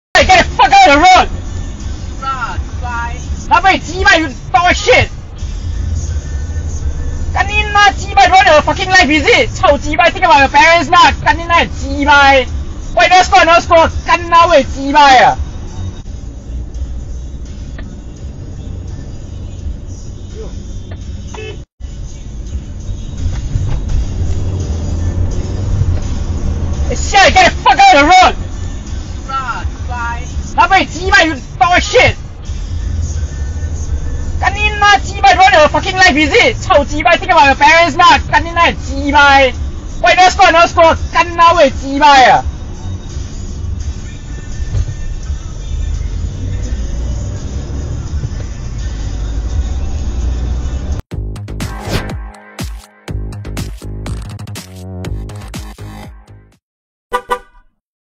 25nov2022 camcar advising cyclist on a fixie without helmet nor brakes not to cycle on the road
25nov2022
Chai Chee
camcar advising cyclist on a fixie without helmet nor brakes fail to conform to red light signal not to cycle on the road
quoted
Young reckless cyclist with bike without brakes dashing thru green light favor traffic. Hope his parents can see this and educate him before some serious accident occurs.